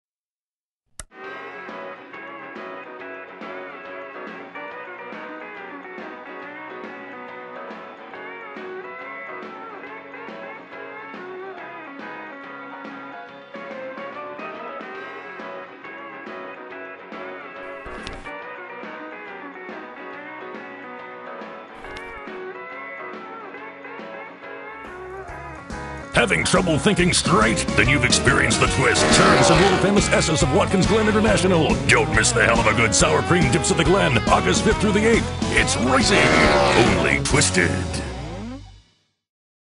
2010 "Esses" TV Commercial: Extended Cake Scene
Enjoy this extended version of the Cake Design scene from the 2010 Heluva Good! Sour Cream Dips at The Glen "Esses" commercial...